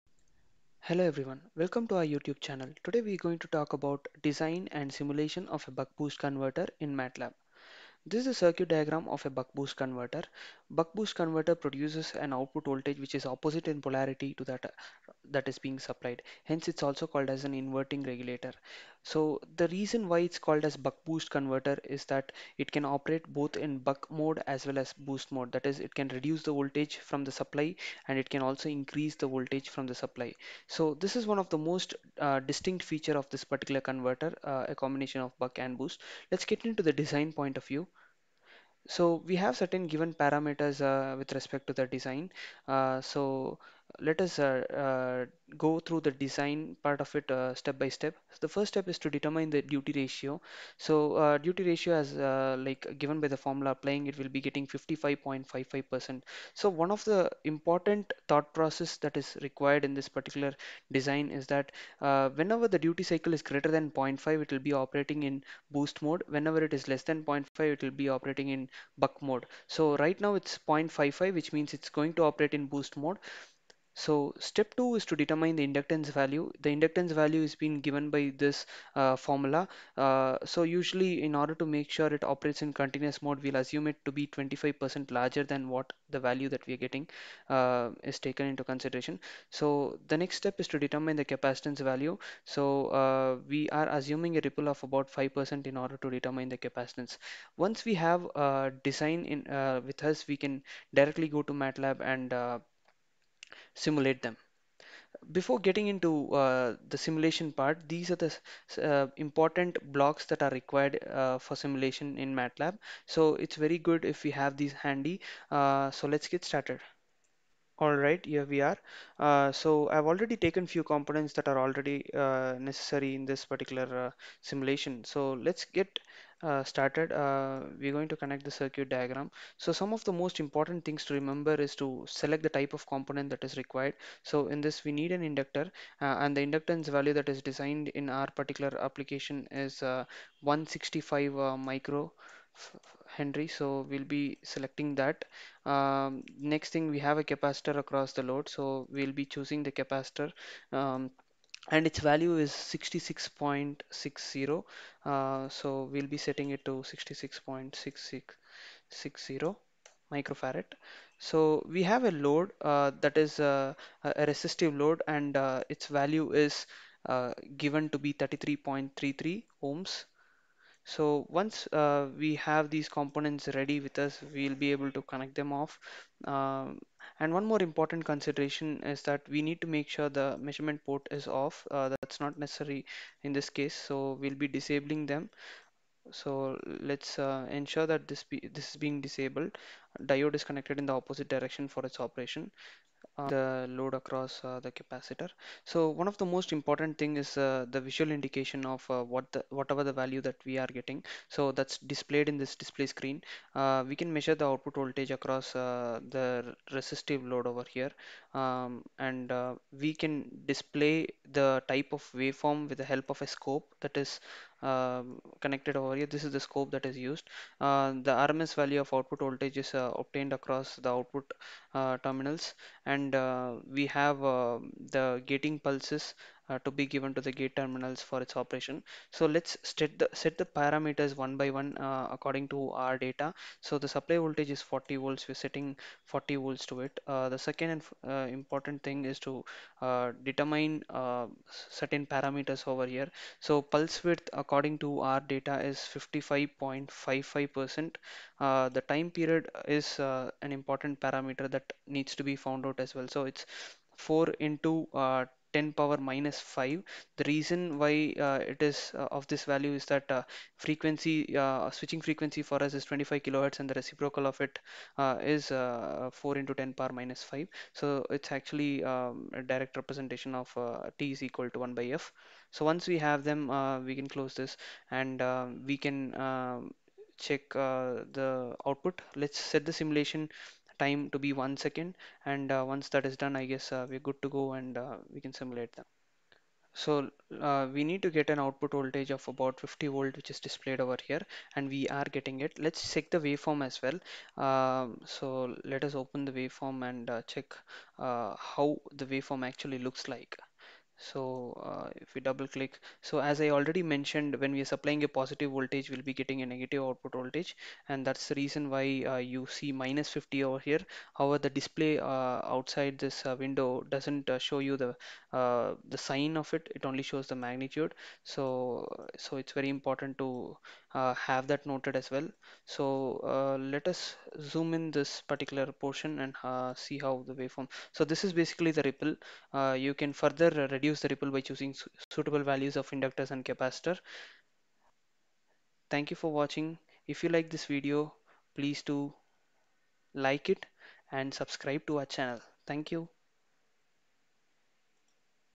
Design and Simulation of Buck-Boost Converter using MATLAB (SIMULINK) / DC-DC Converter
This video demonstrates the design and simulation of the Buck-Boost Converter / DC-DC  Converter using MATLAB/Simulink. The buck-Boost converter can operate as buck regulator or boost regulator. It produces opposite polarity of output voltage from the supply. Hence it is also known as inverting regulator. It plays a very important role in amplifiers and circuits that require negative supply voltage. This video demonstrates the design wrt boost mode only. However, a similar analysis can be applied for designing in buck mode of operation.